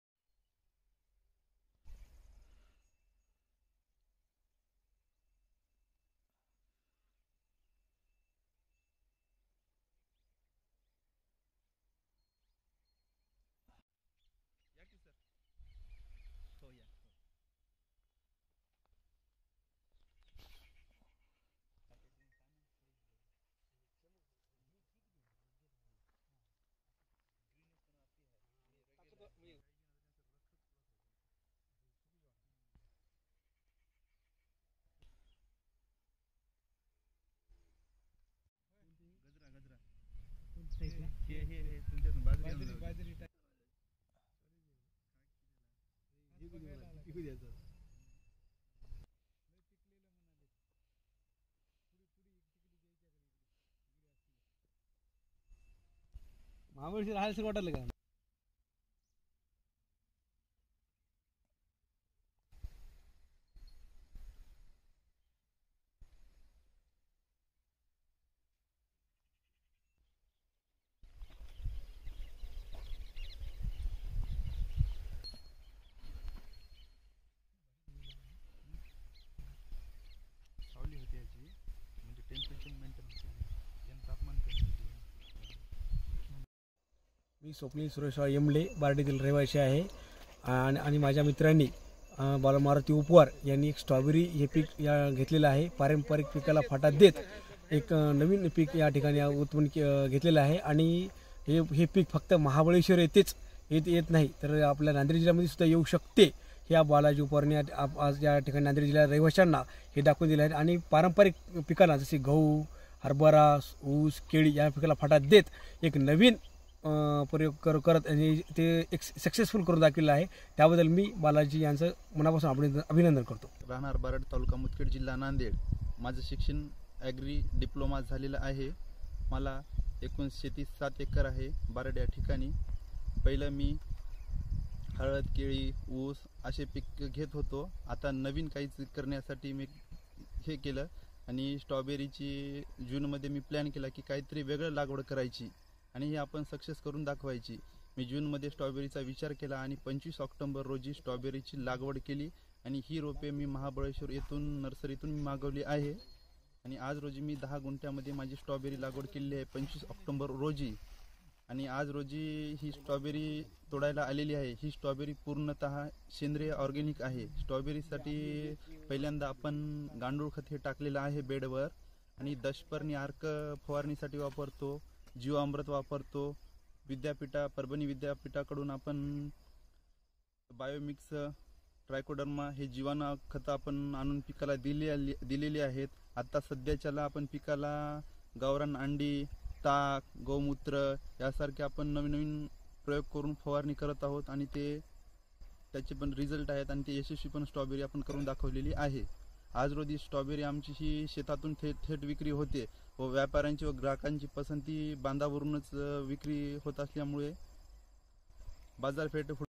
NANDED | नांदेडमध्ये तरुणाने घेतल स्ट्रॉबेरीच उत्पादन
स्ट्रॉबेरी म्हटलं की आठवतं,  थंड हवेचे ठिकाण असलेल्या हिमाचल प्रदेशसह उत्तर भारतातील काही राज्य आणि महाराष्ट्रातील महाबळेश्वर ही ठिकाणे . त्यातच स्ट्रॉबेरी हे पीक फक्त थंड हवेच्या प्रदेशातच पिकतं ,असा सर्वत्र समज आहे ,  परंतु नांदेड जिल्ह्यातील बारड येथील बालाजी उपवार या तरुण शेतकऱ्याने आपल्या शेतात स्ट्रॉबेरीचं यशस्वी उत्पादन घेत एक आगळा वेगळा प्रयोग केला आहे . यामुळे स्ट्रॉबेरीसाठी मराठवाडा वा नांदेडचे वातावरण पोषक नाही असा समज आता दूर झाला आहे.  सध्या बालाजी यांच्या शेतातील  स्ट्रॉबेरी पाहण्यासाठी भोकर , अर्धापूर व परिसरातून लोकांची गर्दी पाहायला मिळते आहे.
मराठवाडा व नांदेड हा परिसर तसा शेतीच्या आणि हवामानाच्या दृष्टीने कोरडवाहू प्रदेश म्हणून ओळखला जातो . पण नवनव्या संशोधनाचा वापर करीत हवामान आणि सेंद्रिय खताचा मूलबक वापर करीत या तरुण शेतकऱ्याने स्ट्रॉबेरीची यशस्वी लागवड केलीय आणि सध्या बालाजी यांच्या शेतात भरपूर स्ट्रॉबेरी लगडलेल्या दिसत आहेत.
या परिसरात प्रामुख्याने हळद, केळी, ऊस, गहू ही पिके मोठ्या प्रमाणात घेतली जातात पण पारंपरिक पिकांना फाटा देत बालाजी यांनी हा प्रयोग यशस्वी करून दाखवला आहे.
 अत्याधुनिक पद्धतीचा उपयोग घेत या तरुण शेतकऱ्याने सेंद्रिय स्ट्रॉबेरी आपल्या शेतात फुलविली आहे ,आता त्यांच्या बांधावर येऊन स्ट्रॉबेरी विक्रेते ३०० किलो प्रमाणे स्ट्रॉबेरी विकत घेत असल्याचे बालाजी उपवार यांनी आमच्या प्रतिनिधींशी बोलताना सांगितले आहे.
आपल्या वडिलोपार्जीत शेतात पारंपारिक पिकांना फाटा देत स्ट्रॉबेरीचे रोपटे पहिल्यांदा महाबळेश्वर येथील रोपवाटीकेतून आणले , त्यांनी स्ट्रॉबेरीच्या एस.ए. या जातीच्या वाणाची निवड केली.
प्रारंभी दहा गुंठे जमिनीवर अत्याधुनिक पद्धतीचा वापर करीत पाच फुटाचा बेड करून त्यात गांडूळ खताचा व सेंद्रिय खताचा वापर करीत यात जीवामृत दशपर्णी आर्क, डी कंपोजर , आणि परभणीच्या कृषी विद्यापीठातून सूक्ष्म जिवाणू आणून पिकांना दिले आहेत, तर बेडवरती मलचिंग पेपरचा वापर केला असल्याचेही बालाजी उपवार यांनी सांगितले आहे.
स्ट्रॉबेरी या पिकाला थंड वातावरणाची आवश्यकता असते , पीक घेतलेल्या परिसरात थंड वातावरण कायम ठेवण्यासाठी स्प्रिंकलरचा सातत्याने उपयोग केला आहे, सोबतच ड्रीपच्या साह्याने दररोज वीस मिनिटे पाणी दिले , आणि पिकाची रोग प्रतिकार क्षमता वाढविण्यासाठी गोमूत्र, ताक , गावरान अंडी या घटकांचे मिश्रण करून पिकावर फवारणी केली आहे . यासोबतच पाणी देण्याच्या वेळा देखील नियमित काटेकोरपणे पाळल्याचे तरुण शेतकरी बालाजी उपवार यांनी आमच्याशी बोलताना सांगितले आहे.